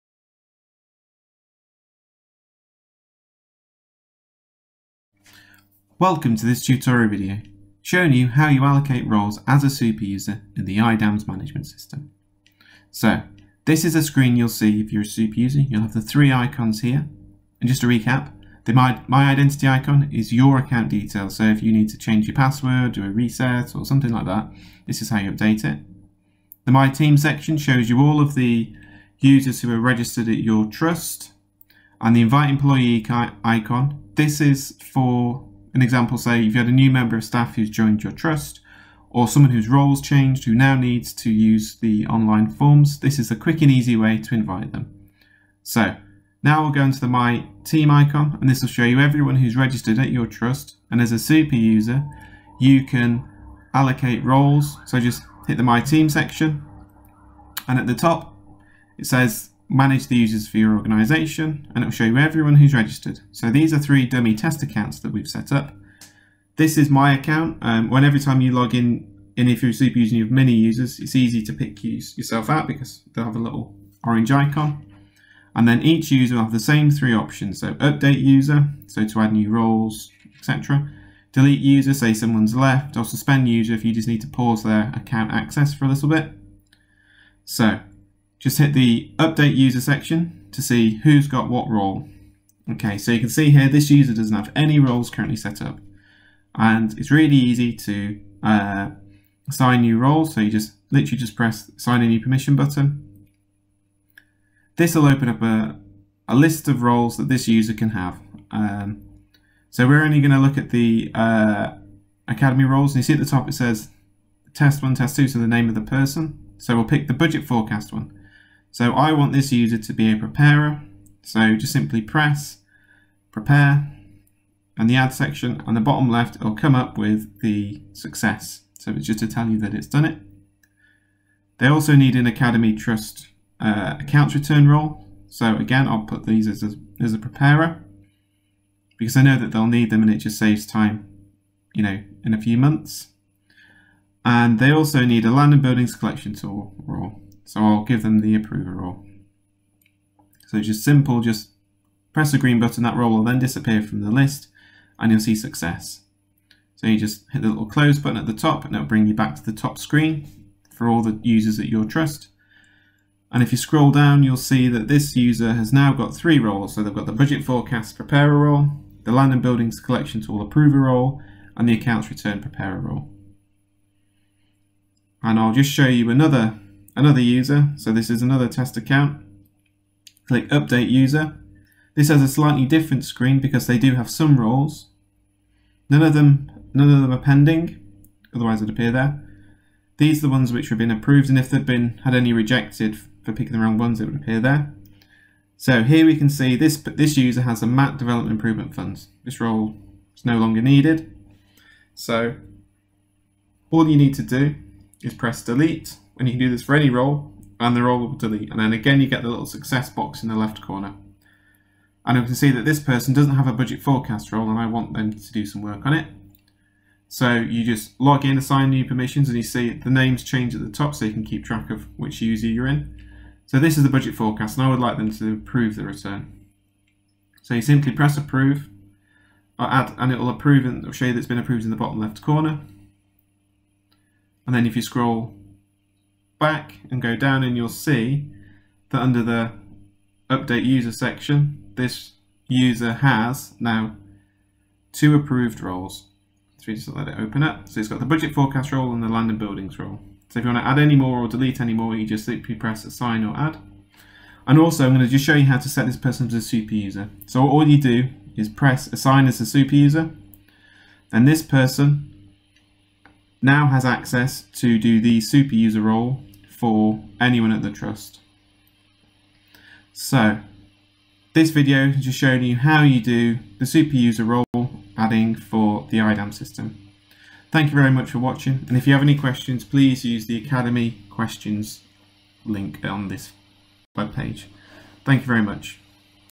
How to assign roles in the IDAMS system
This video explains how to assign roles in the IDAMS system for the accounts return and land and buildings collection tool.